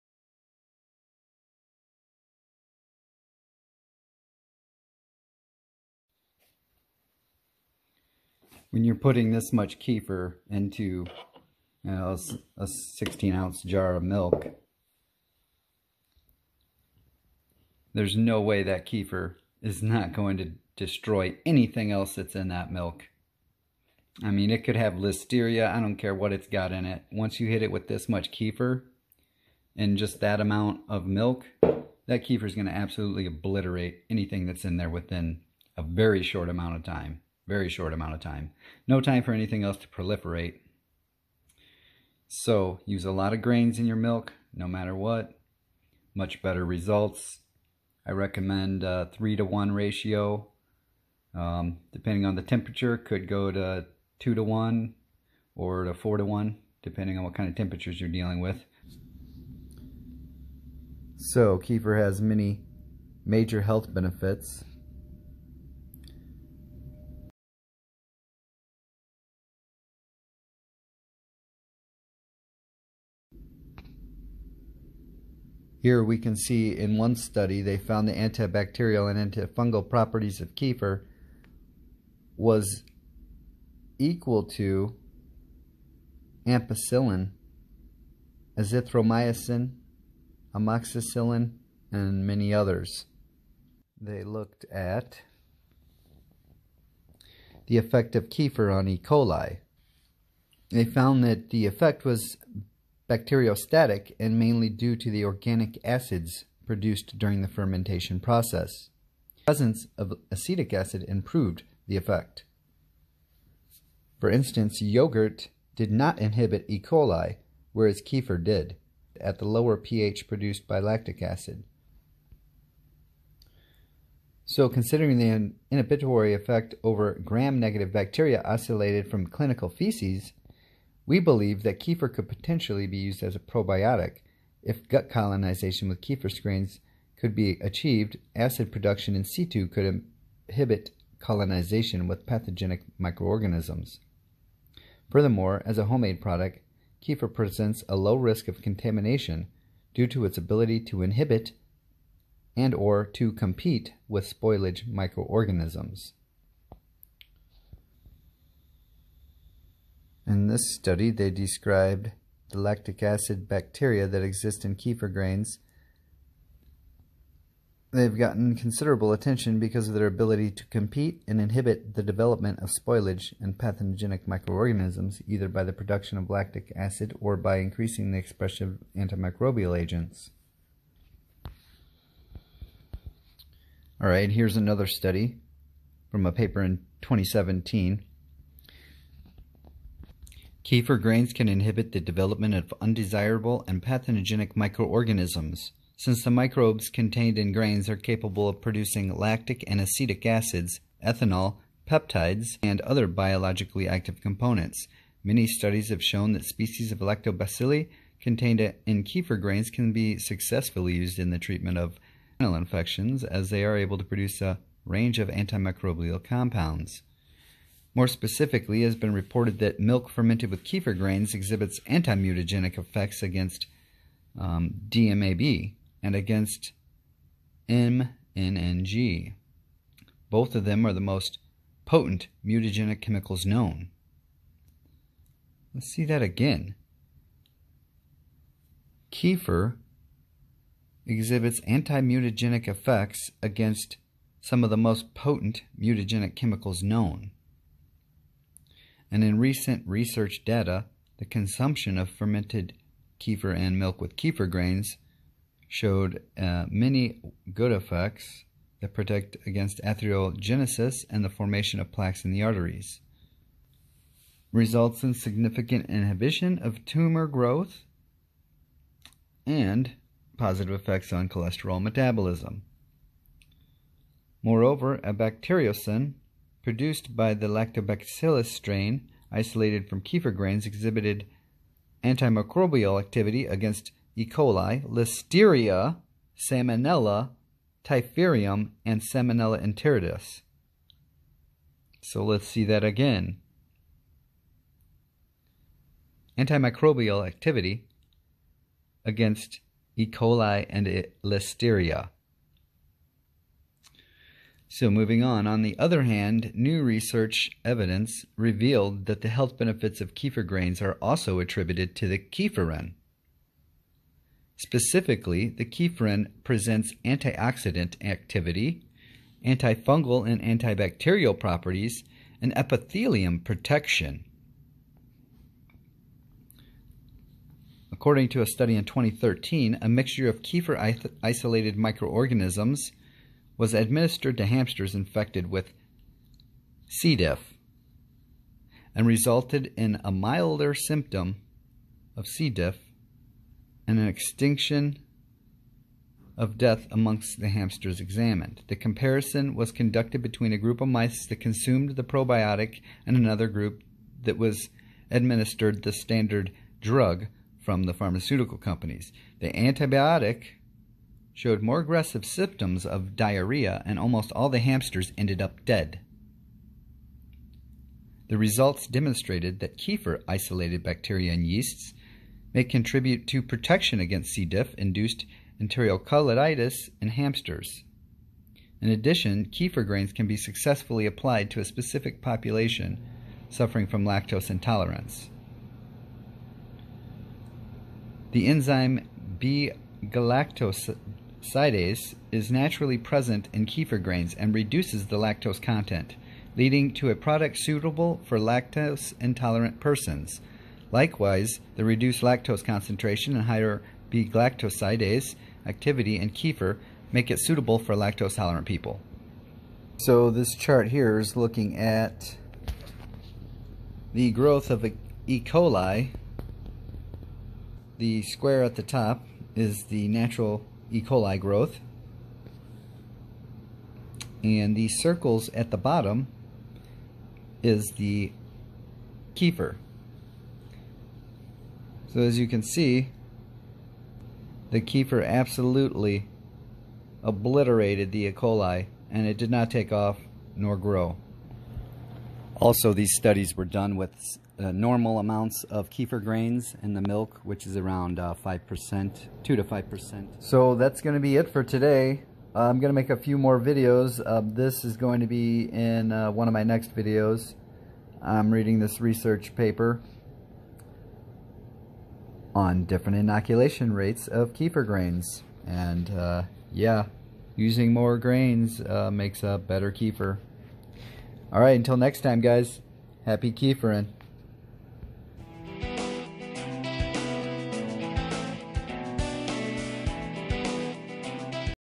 Kefir Science Power of the Grains Homemade Milk Kefir Safety/ it's Bacteriostatic
If just 5% kefir stops ecoli, Listeria, and Salmonella, imagine what 30% , 1/3 kefir grain would do to it...

increased safety with all kinds of milk kefir by ensuring a quick and complete Innoculation.

I use Raw Milk

see also -

R/kefir on reddit

For information and sourcing of raw milk in your area see -